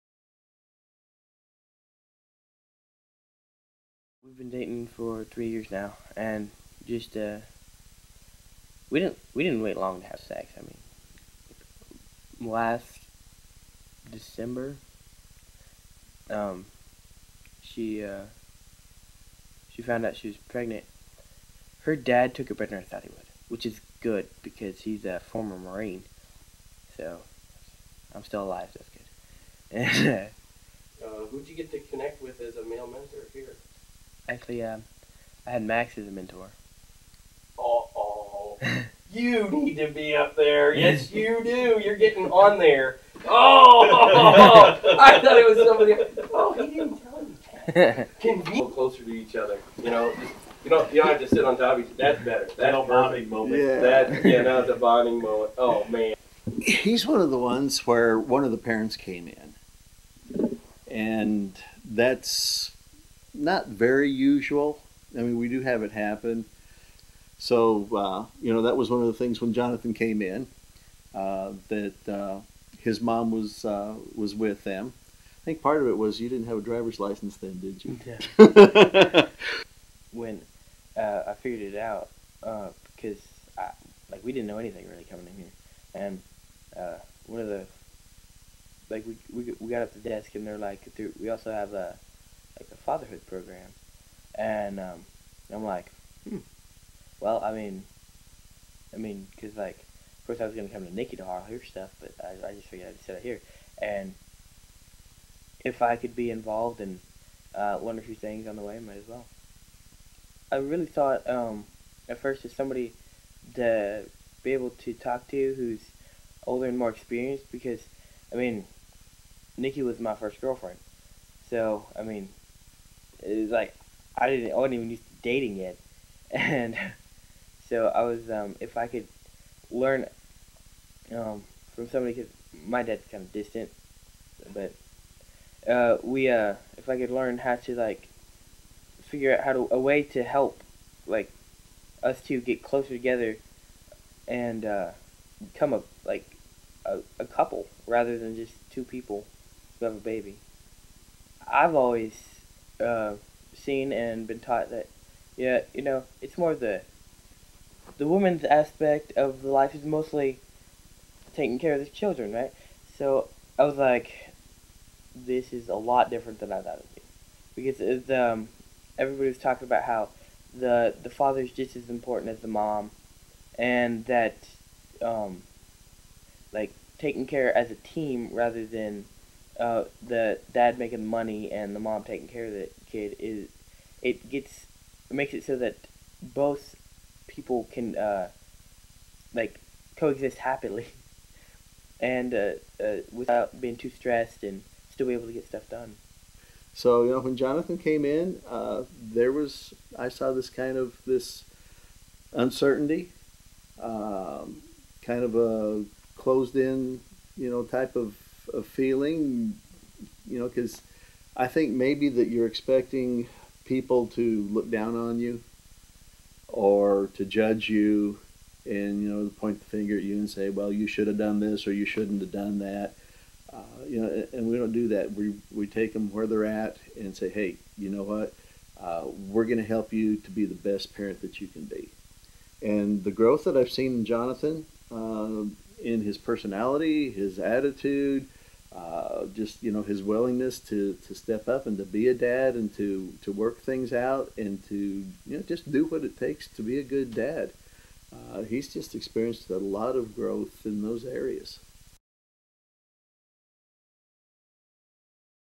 John and his mentor Max
John describes time with his mentor Max